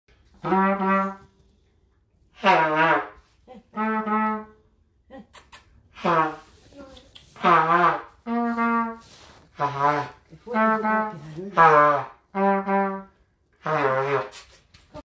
Practice to play trumpet and the dog like.
While they practice to play trumpet the dog want to play also.